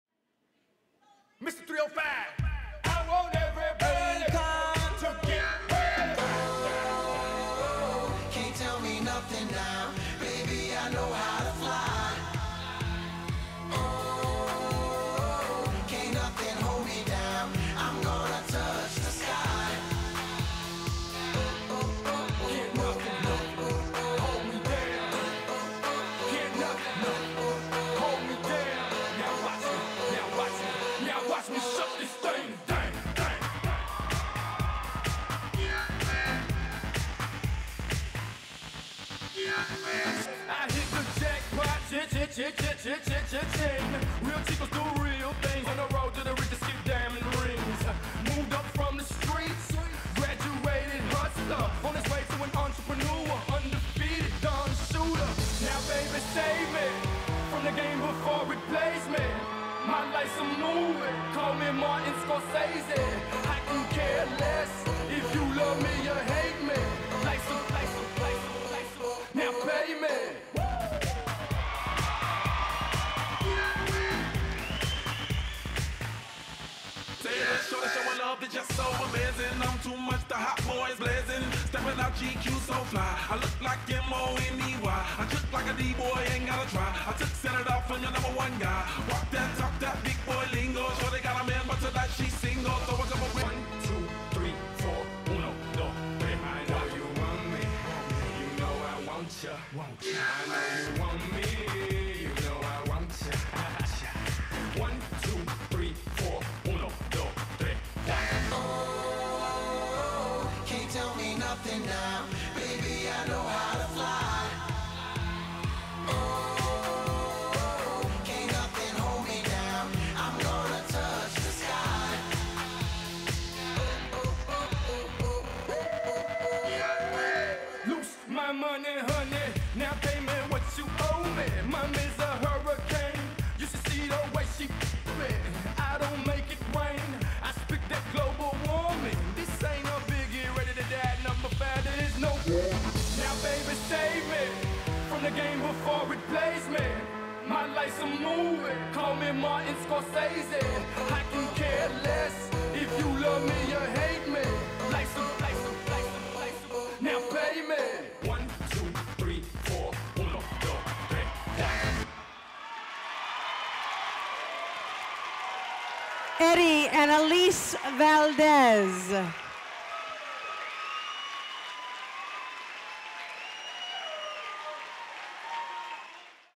Pro Showcase 1st Place - 2011 Americas' Classic
Winning performance in the Pro Showcase at 2011 Americas Classic Championships. Houston Tx. January 13-16, 2011.

Footage courtesy of Doug Silton Productions • Los Angeles, CA.